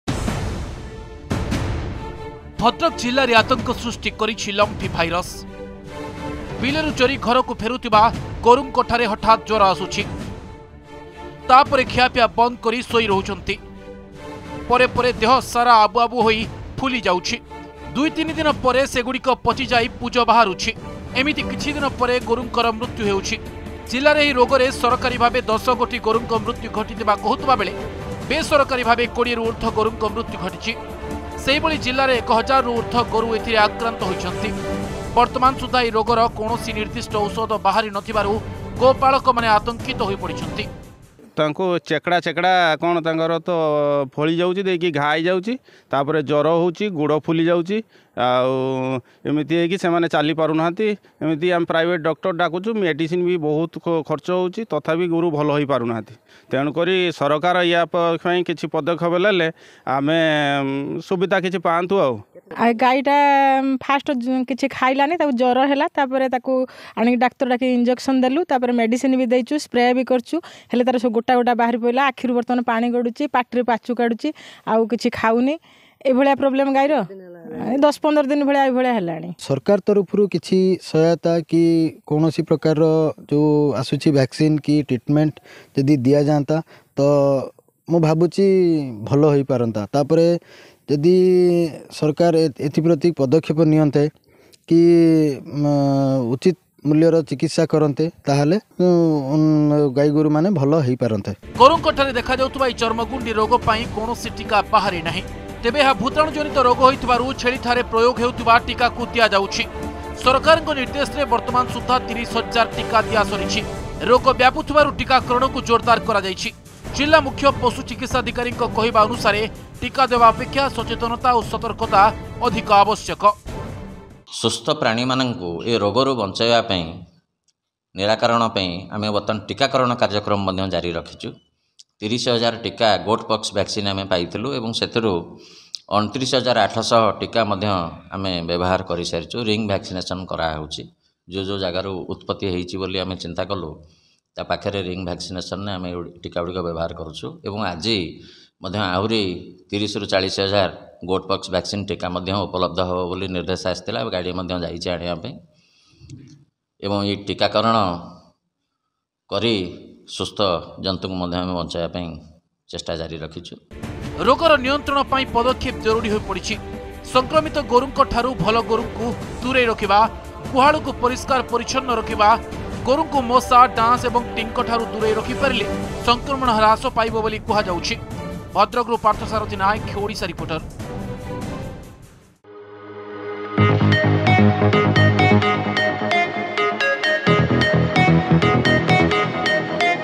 ଚର୍ମଗୁଣ୍ଡି ରୋଗକୁ ନେଇ ଆତଙ୍କ | Odisha Reporter
ଭଦ୍ରକ ଜିଲ୍ଳାରେ ଆତଙ୍କ ସୃଷ୍ଟି କରିଛି ଲମ୍ଫି ଭାଇରସ । ବିଲରୁ ଚରିବୁଲି  ଘରକୁ ଫେରୁଥିବା ଗୋରୁଙ୍କଠାରେ ହଠାତ ଜ୍ବର ଆସୁଛି। ତା ପରେ ଖିଆପିଆ ବନ୍ଦ କରି ଶୋଇ ରହୁଛନ୍ତି। ପରେ ପରେ ଦେହ ସାରା ଆବୁ ଆବୁ ହୋଇ ଫୁଲି ଯାଉଛି।